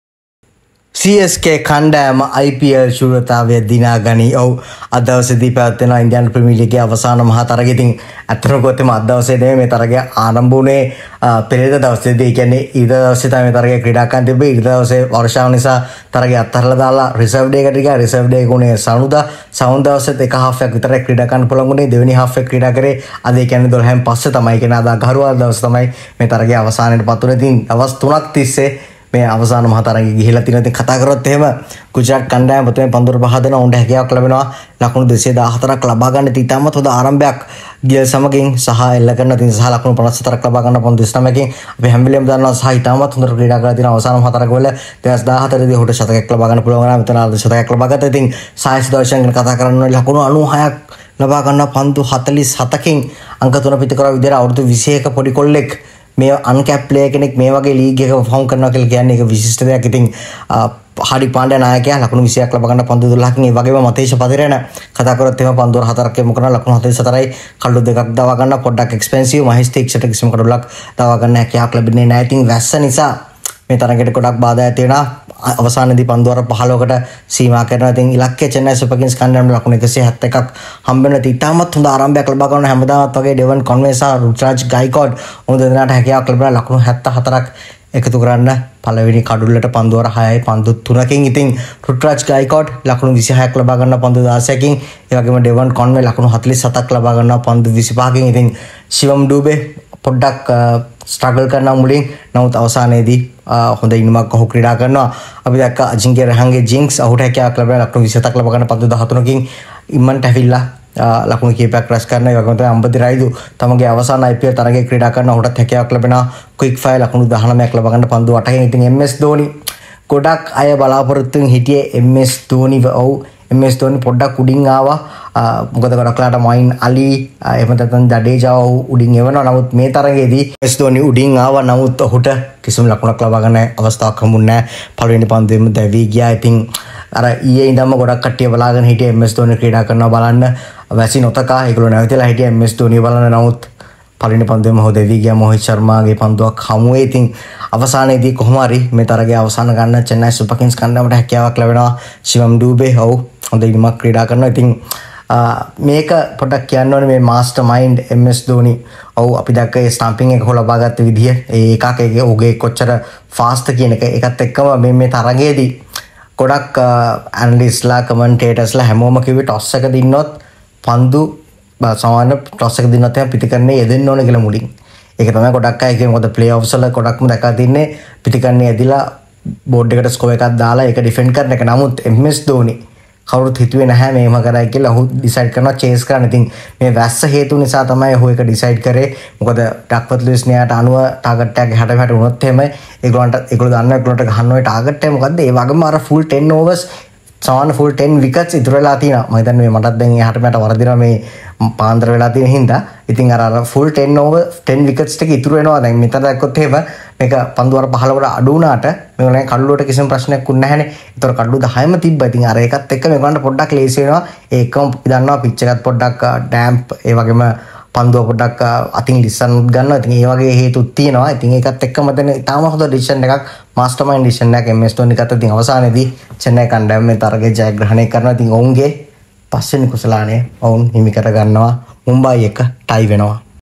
Ravindra Jadeja hands Chennai Super Kings IPL 2023 beating Gujarat Titans
Chennai Super Kings beat Gujarat Titans to win IPL 2023